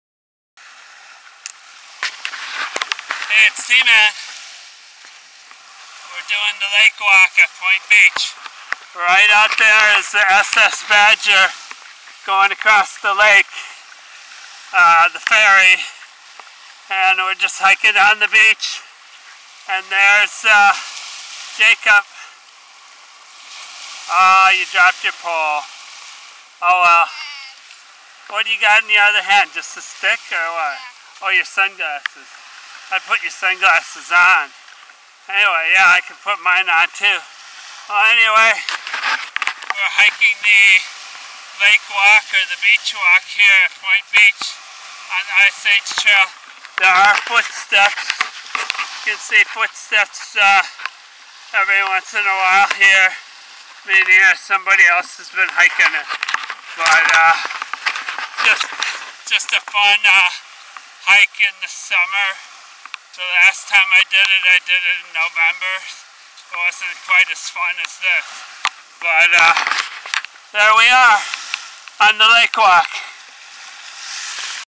Tman and Jacob do the Point Beach IAT Lakewalk
Hiking along Lake Michigan on the Ice Age Trail